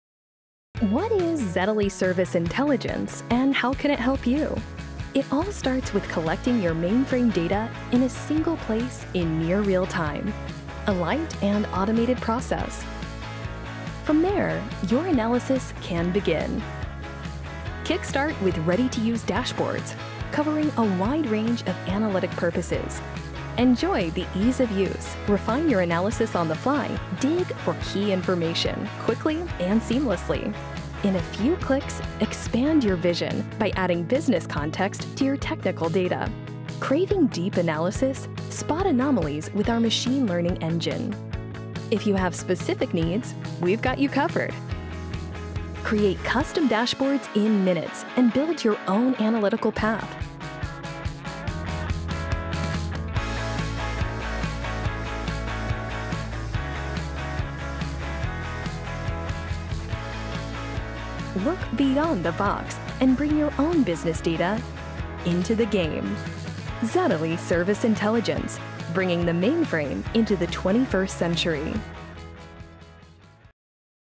Zetaly Service Intelligence - Get mainframe insights at your fingertips!
Zetaly Service Intelligence (SI) is a Mainframe Operations Analytics solution designed for simplicity. It offers the opportunity to leverage operational data for system performance optimization.

Easy to use, even by non-experts, it bridges the gap between infrastructure and business.

- Gain visibility into your mainframe activity
- Understand the dependencies of your infrastructure
- Connect technical data to its business context
- Track critical key performance indicators and detect anomalies